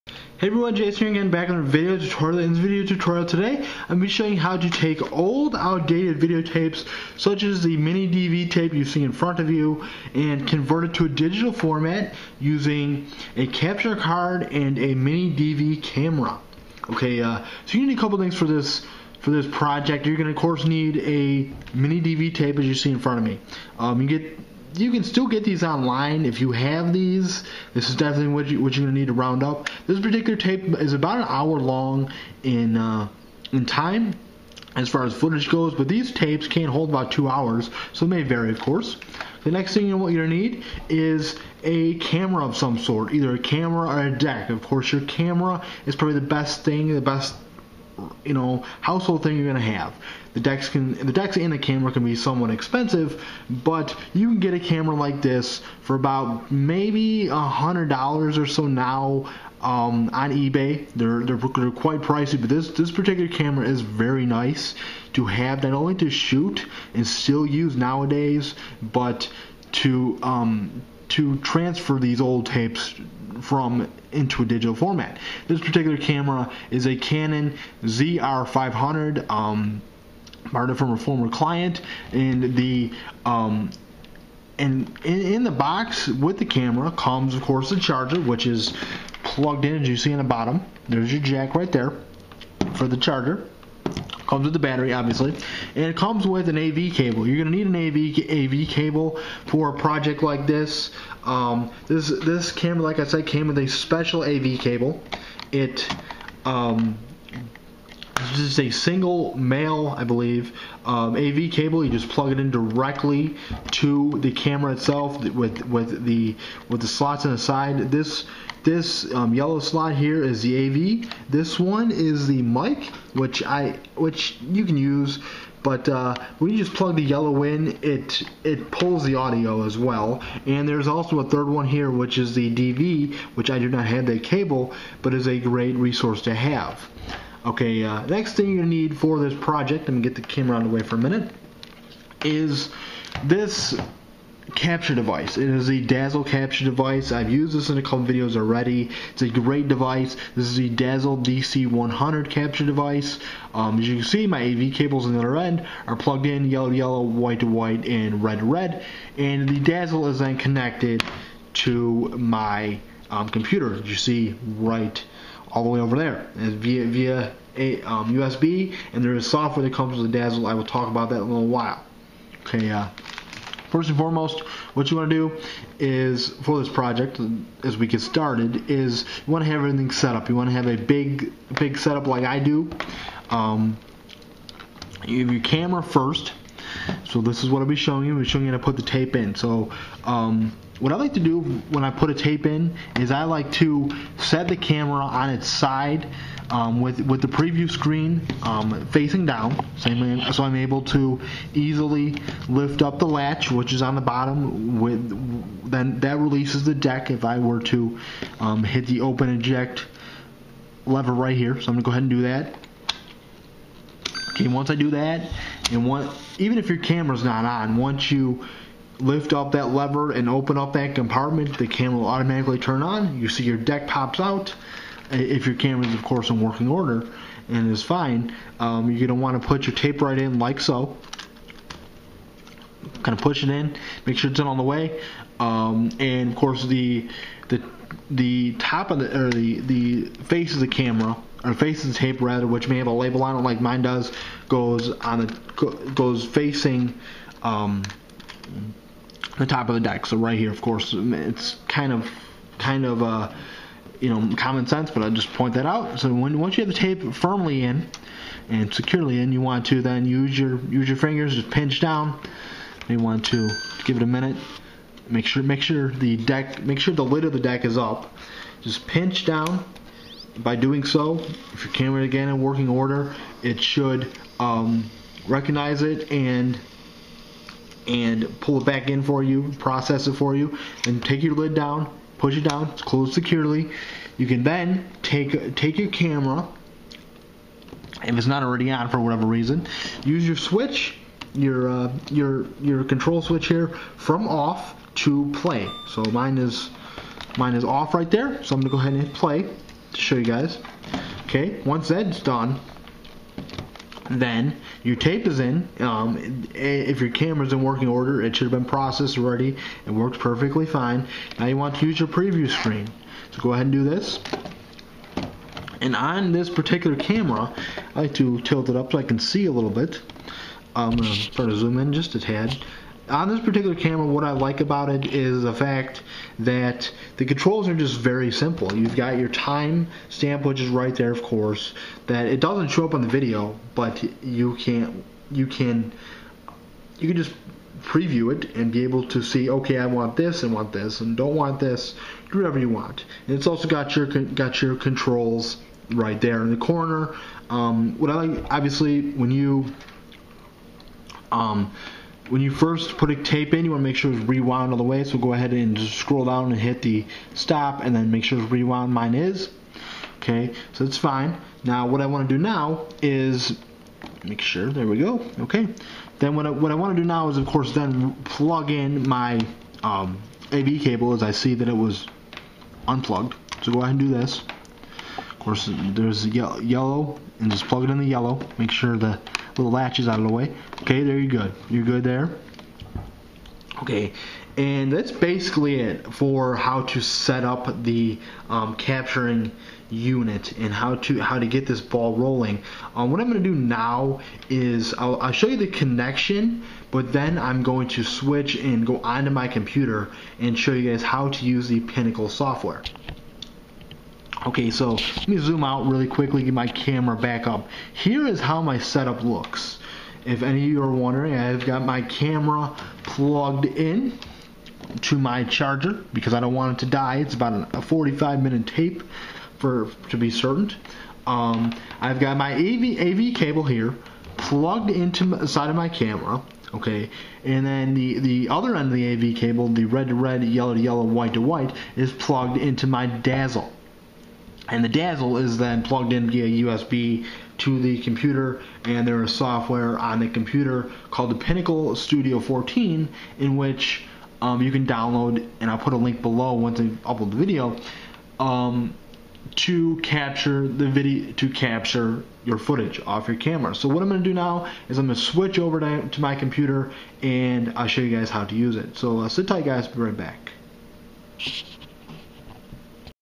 How To Transfer MiniDV Tapes To A Computer/Digital Format: Part 1
In this video tutorial I explain how to transfer MiniDV tapes to a computer.